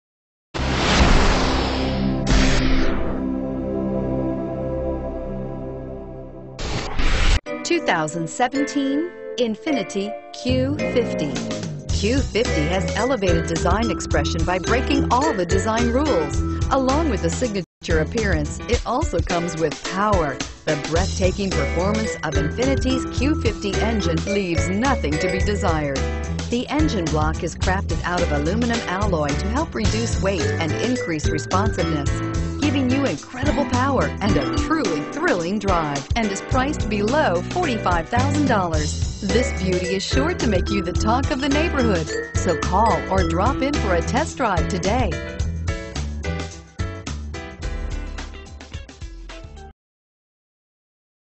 2017 INFINITI Q50 Dallas Ft. Worth Grapevine 32761
Grubbs INFINITI
1500 E State Highway 114

* 3 LITER 6 Cylinder engine * * 2017 ** INFINITI * * Q50 * * 3.0T Premium * Featuring a push button start, a backup camera, Bluetooth, braking assist, dual climate control, a power outlet, hill start assist, traction control, Bose sound system, and a premium sound system, be sure to take a look at this 2017 INFINITI Q50 3.0T Premium before it's gone. It has a beautiful chestnut bronze exterior and a c/wheat interior. It has the best mileage with 20 MPG in the city and 29 MPG on the highway. Call today to take this one out for a spin!